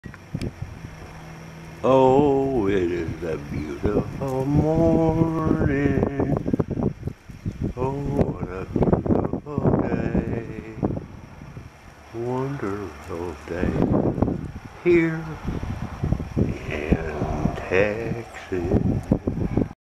We haven't seen sun in weeks now today it is beautiful at 7:30 AM.